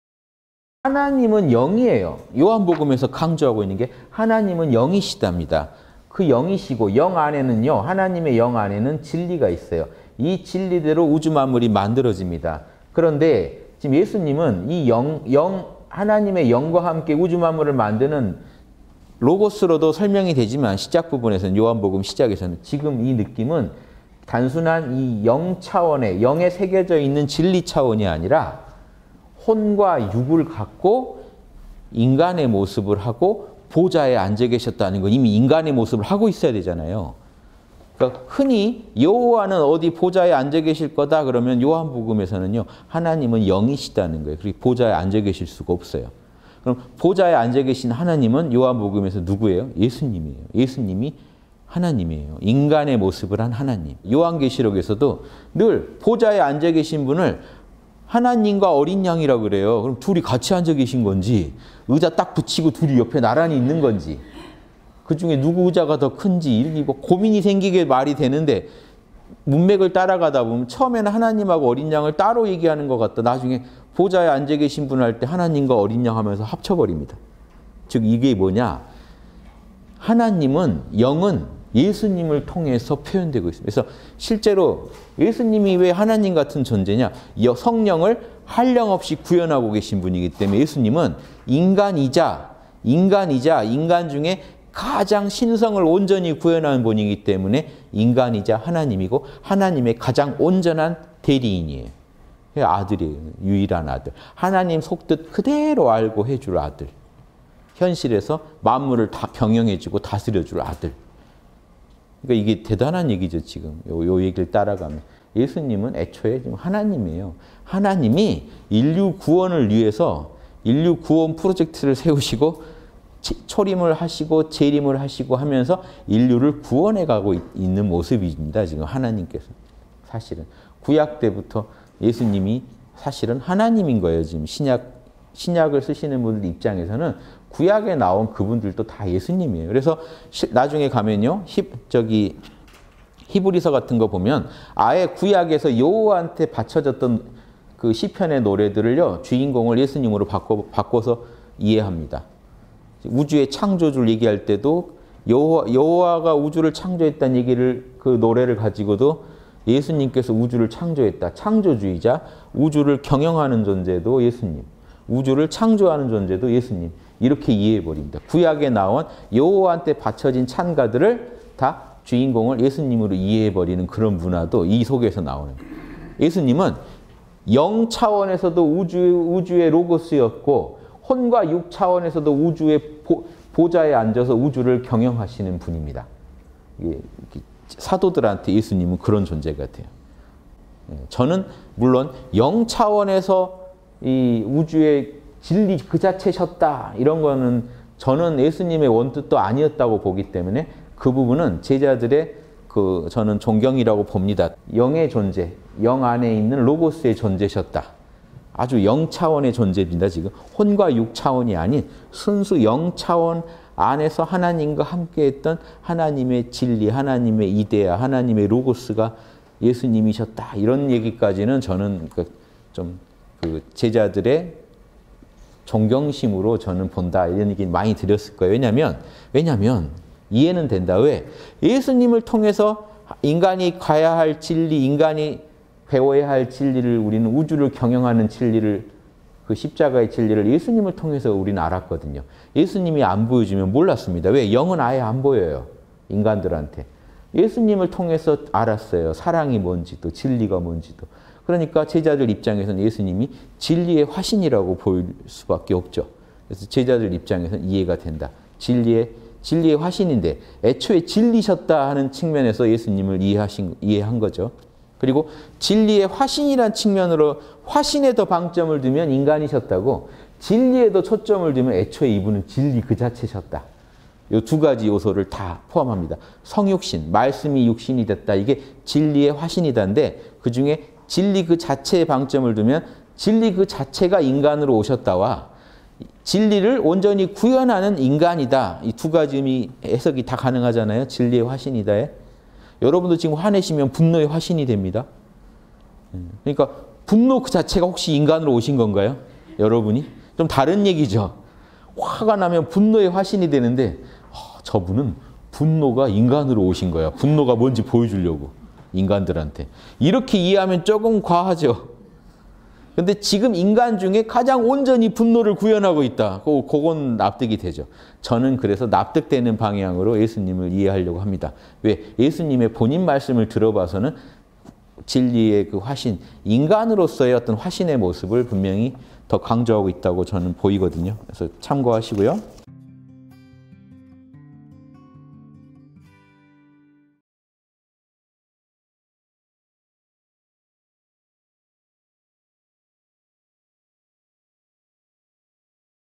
[3분 묵상] 성육신의 이해(말씀이 육신이 되다)_홍익학당.윤홍식
(190228 요한복음) 하느님은 영이에요.
요한복음에서 강조하고 있는 게,
'하느님은 영이시다!'입니다.
그 영이시고, 영 안에는요, 하느님의 영 안에는
진리가 있어요. 이 진리대로 우주 만물이 만들어집니다.

그런데 지금 예수님은 이 영, 하느님의 영과 함께
우주 만물을 만드는 로고스로도 설명이 되지만
시작 부분에서는, 요한복음 시작에서는요.

지금 이 느낌은 단순한 이 영 차원의,
영에 새겨져 있는 진리 차원이 아니라,
혼과 육을 갖고, 인간의 모습을 하고 보좌에 앉아 계셨다는 건
이미 인간의 모습을 하고 있어야 하잖아요?

흔히 "여호와는 어디 보좌에 앉아 계실 거다!"라고 그러면
요한복음에서는요, '하느님은 영이시다!'라는 거예요.
그렇게 보좌에 앉아 계실 수가 없어요.

그럼 보좌에 앉아계신 하느님은 요한복음에서는 누구예요?
예수님이에요. 예수님이 하느님이에요.
인간의 모습을 한 하느님.

요한계시록에서도, 늘 보좌에 앉아계신 분을
'하느님과 어린 양'이라고 그래요.
그럼 둘이 같이 앉아 계신 건지?
의자 딱 붙이고 둘이 옆에 나란히 있는 건지?
그중에 누구 의자가 더 큰지?
이런 고민이 생기게 말이 되는데,
문맥을 따라가다 보면 처음에는 '하느님하고 어린 양'을
따로 얘기하는 것 같다가 나중에는
'보좌에 앉아계신 분'할 때,
'하느님과 어린 양'하면서 합쳐버립니다.

즉, 이게 뭐냐?
하느님은(영은) 예수님을 통해서 표현되고 있습니다.
그래서 실제로 예수님이 왜 하느님 같은 존재냐?
성령을 한량없이 구현하고 계신 분이기 때문에
예수님은 인간이자, 인간 중에 가장 신성을 온전히
구현한 분이기 때문에 인간이자 하느님이고,
하느님의 가장 온전한 대리인이에요.

그게 아들이에요, 유일한 아들.
하느님 속뜻 그대로 알고 해줄 아들.
현실에서 만물을 다 경영해주고 다스려줄 아들.
그러니까 이게 대단한 얘기죠, 지금.
이 얘기를 따라가면.

예수님은 애초에 하느님이에요.
하느님이 인류 구원을 위해서 인류 구원 프로젝트를 세우시고,
초림을 하시고, 재림을 하시고 하면서
인류를 구원해가고 있는 모습입니다, 지금.
하느님께서, 사실은.

구약 때부터 예수님이 사실은 하느님인 거예요.
지금 신약을 쓰시는 분 입장에서는,
구약에 나오신 그분들도 다 예수님이에요.

그래서 나중에 가면요, '히브리서' 같은 거 보면
아예 구약에서 여호와께 바쳐졌던 시편의 노래들을요,
주인공을 예수님으로 바꿔서 이해합니다.

우주의 창조주를 얘기할 때도,
여호와가 우주를 창조했다는 얘기를, 그 노래를 가지고도
예수님께서 우주를 창조했다,
창조주이자 우주를 경영하는 존재도 예수님,
우주를 창조하는 존재도 예수님,
이렇게 이해해버립니다.

구약에 나온 여호와한테 바쳐진 찬가들을,
다 주인공을 예수님으로 이해해버리는 그런 문화도
이 속에서 나오는 거예요.

예수님은 영 차원에서도 우주의 로고스였고,
혼과 육 차원에서도 우주의 보좌에 앉아서
우주를 경영하시는 분입니다.
사도들한테 예수님은 그런 존재 같아요.

저는 물론 영 차원에서 '이 우주의 진리 그 자체셨다!' 이런 거는,
저는 예수님의 원뜻도 아니었다고 보기 때문에
그 부분은 제자들의 존경이라고 봅니다.

"영의 존재, 영 안에 있는 로고스의 존재셨다."
아주 영 차원의 존재입니다, 지금.
"혼과 육 차원이 아닌, 순수 영 차원 안에서
하느님과 함께했던 하느님의 진리, 하느님의 이데아,
하느님의 로고스가 예수님이셨다!" 이런 얘기까지는
저는 좀 제자들의 존경심으로 저는 본다고
이 얘기는 많이 드렸을 거예요.

왜냐하면, 이해는 됩니다.
왜? 예수님을 통해서 인간이 가야 할 진리,
인간이 배워야 할 진리를, 우리는 우주를 경영하는 진리를,
그 십자가의 진리를, 예수님을 통해서 우리는 알았거든요.
예수님이 안 보여주면 몰랐습니다. 왜?
영은 아예 안 보여요, 인간들한테.
예수님을 통해서 알았어요. 사랑이 뭔지도, 진리가 뭔지도.

그러니까 제자들 입장에서는 예수님이
'진리의 화신'이라고 보일 수밖에 없죠.
제자들 입장에서는 이해가 됩니다.

'진리의 화신인데 애초에 진리셨다.' 하는 측면에서
예수님을 이해한 거죠.
그리고 '진리의 화신'이란 측면으로
화신에다 방점을 두면 '인간이셨다.'고,
진리에 더 초점을 두면 '애초에 이분은 진리 그 자체셨다.'
이 두 가지 요소를 다 포함합니다.

성육신(成肉身), '말씀이 육신이 되었다.'
이게 '진리의 화신이다.'인데
그중에 진리 그 자체에 방점을 두면
'진리 그 자체가 인간으로 오셨다.'와
'진리를 온전히 구현하는 인간이다.' 이 두 가지 의미의 해석이
다 가능하잖아요, '진리의 화신이다.'에.

여러분도 지금 화내시면 '분노의 화신'이 됩니다.
그러니까 분노 그 자체가 혹시 인간으로 오신 건가요, 여러분이?
좀 다른 얘기죠.
화가 나면 '분노의 화신'이 되는데,
저분은 분노가 인간으로 오신 거야.
분노가 무엇인지 보여주려고, 인간들한테.
이렇게 이해하면 조금 과하죠.

그런데 지금 인간 중에 가장 온전히 분노를 구현하고 있다.
그건 납득이 되죠.
저는 그래서 납득되는 방향으로 예수님을 이해하려고 합니다.
왜? 예수님의 본인 말씀을 들어봐서는
진리의 화신, 인간으로서의 어떤 화신의 모습을
분명히 더 강조하고 있다고 저는 보이거든요.
그래서 참고하시고요.